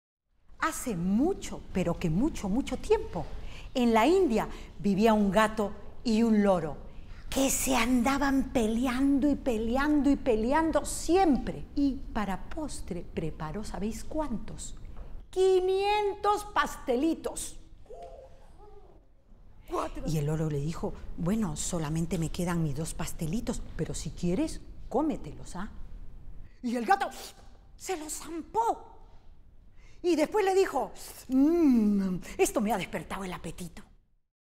El gato y el loro
Hace mucho, mucho, pero que mucho tiempo, en la India vivían un gato y un loro...
Narradora: Mercedes Carrión
Autor original: Cuento popular hindú